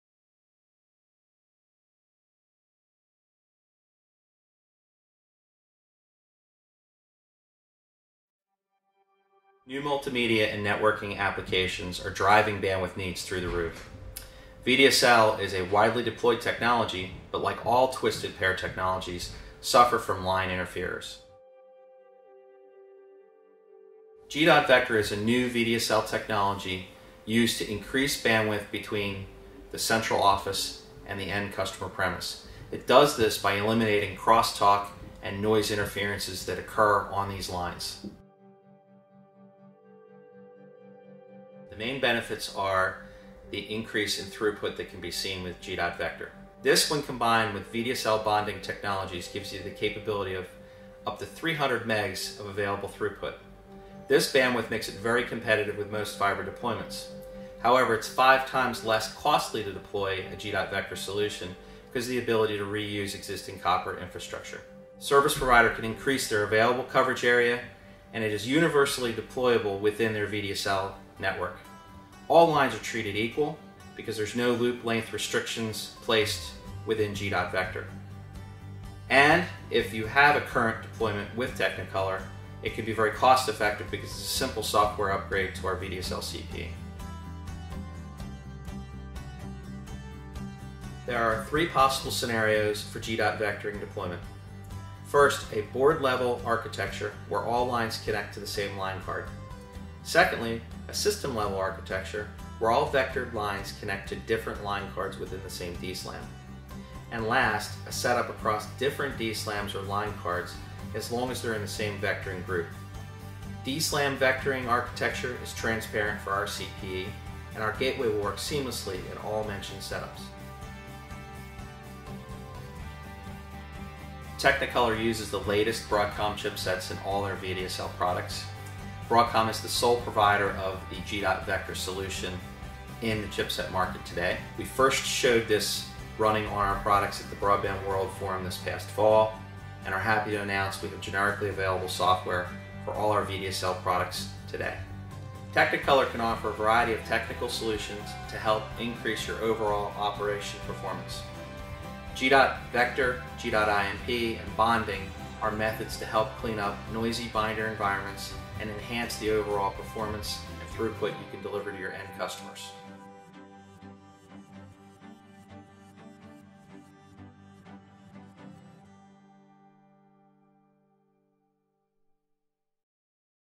G.Vector - A New VDSL2 Technology
Technicolor a frontrunner in VDSL Vectoring explains G.Vector, a new VDSL2 technology standard that increases bandwidth and reach from the cabinet to the home or premises further than traditional VDSL2 solutions by cancelling the crosstalk noise inherently present in VDSL2 bands.